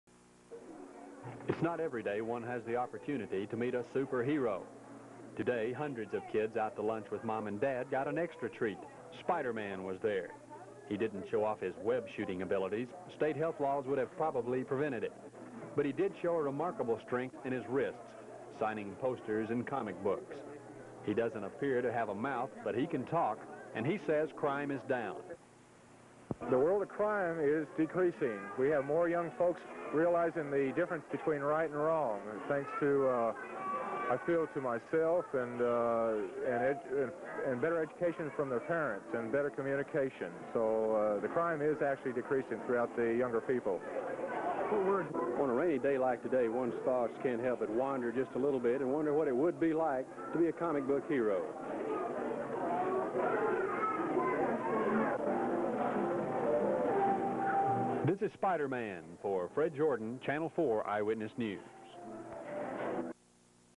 Fred Jordan 1978 Spiderman
1978 - KARK-TV Little Rock  Reporter-Fred Jordan, Photographer-Paul M. Summitt